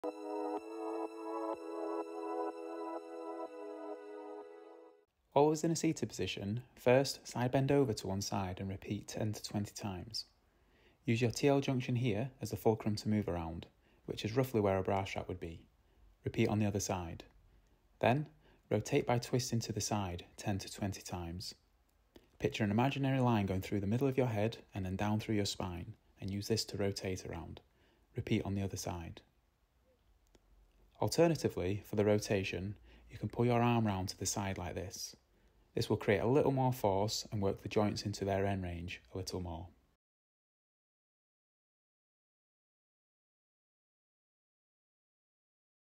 84. T/L Sidebends & Rotations - Mobility Exercise for Back Pain
This is a simple exercise used to mobilise the T/L junction in your spine, which is where the Thoracic Spine meets the Lumbar Spine and sits just beneath where a bra strap would be.

Not many people present with pain here, however this junction is an area of the spine that is susceptible to large amounts of mechanical load and it is common for this area of the spine to become stiff and contribute to Lower Back pain as the Lower Back compensates.

In turn, the lumbars take up the strain as the T/L isn’t moving as it should which may lead to pain in the lower back rather than the T/L junction.

The cause of an issue is not always where the pain is.

Suggested reps: 10-20reps both sides. 3-4 sets